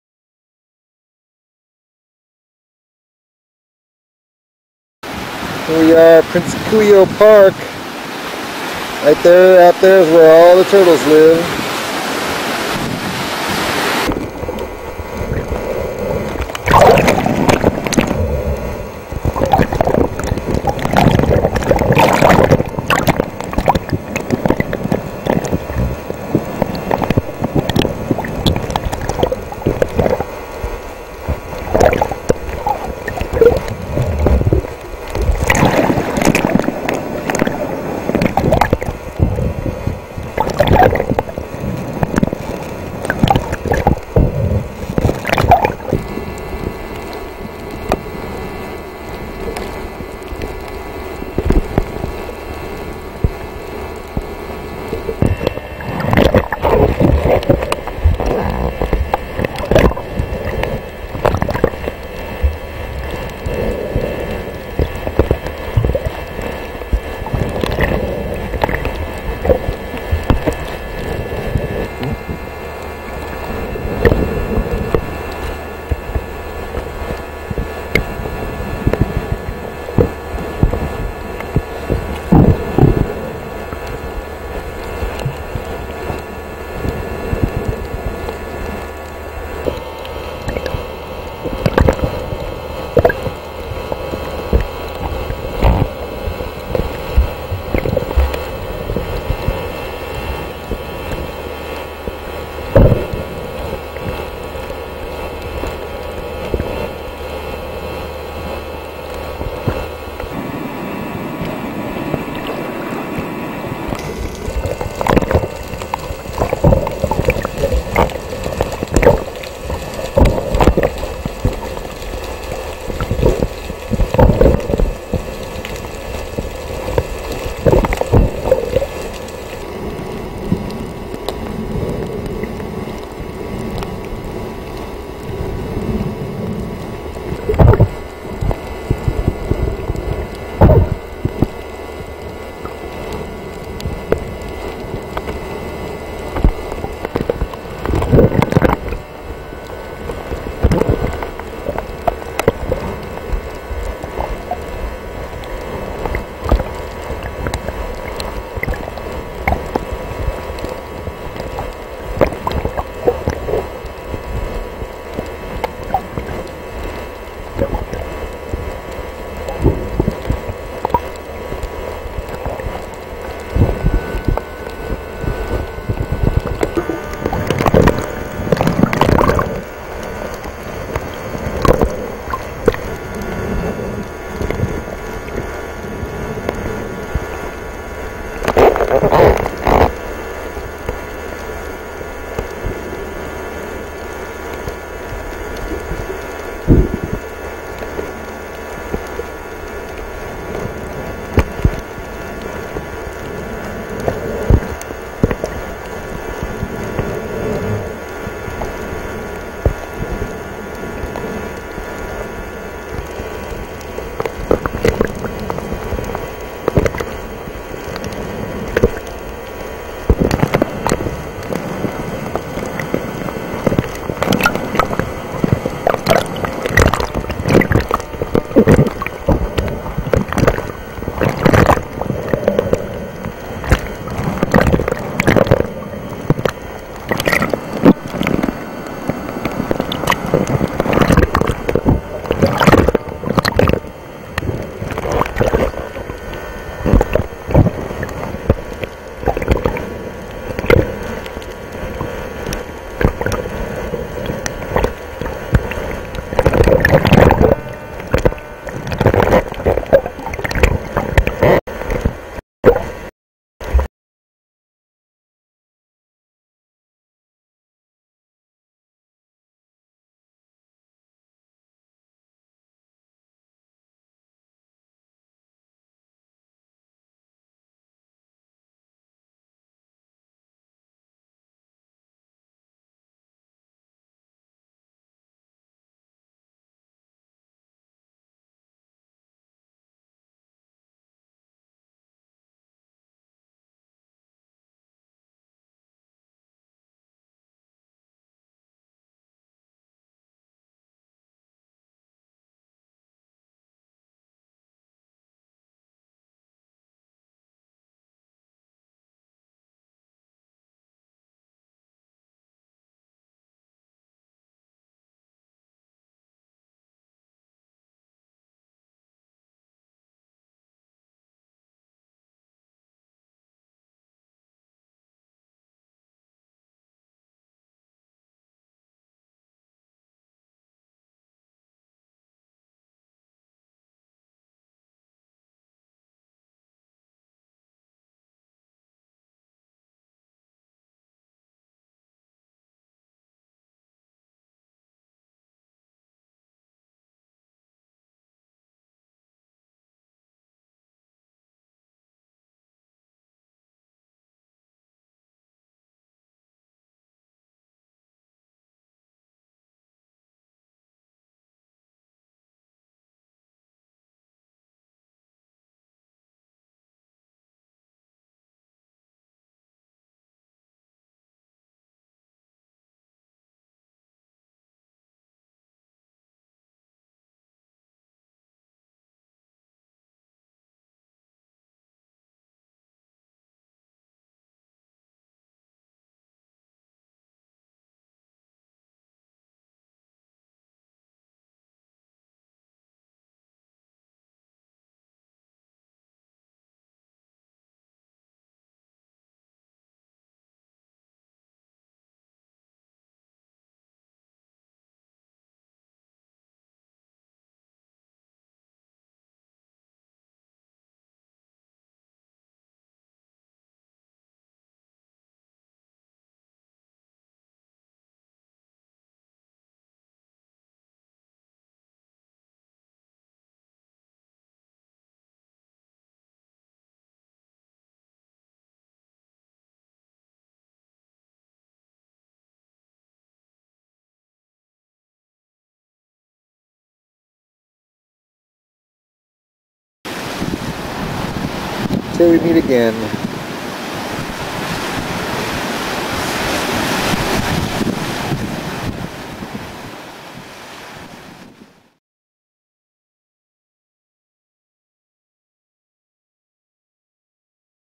Swimming with Sea Turtles
Swimming with green sea turtles off the coast of Kauai, Hawaii